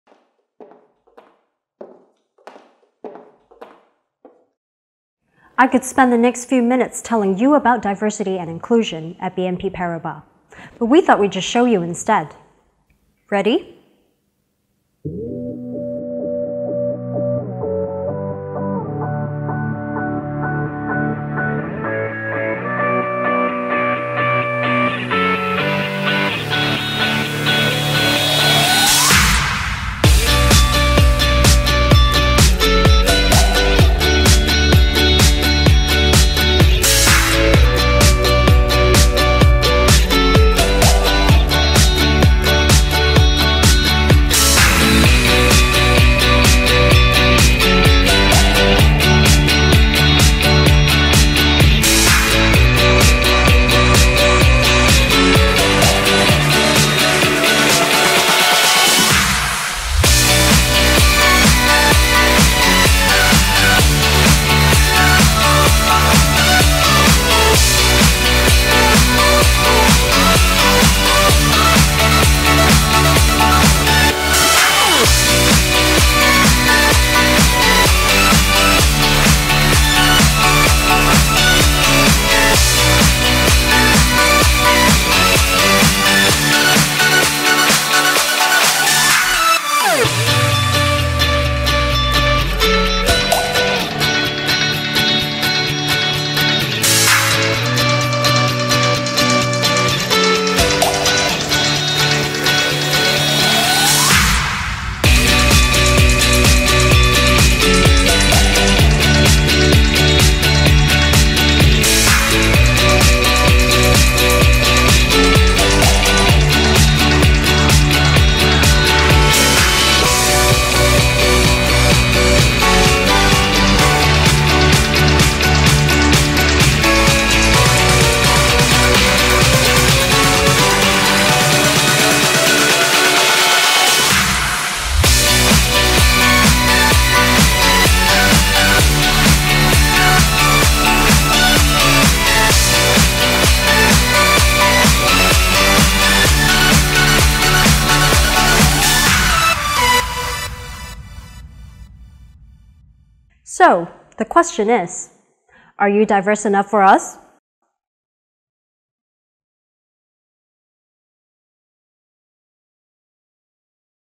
BNP Paribas - Diversity & Inclusion Film (With Simplified Chinese Caption)
At BNP Paribas they believe that building a diverse talent pool is critical to our future success. They are committed to creating a diverse and inclusive culture, to promote APAC talent and to drive gender diversity. This video shares about what it is like to work at BNP Paribas.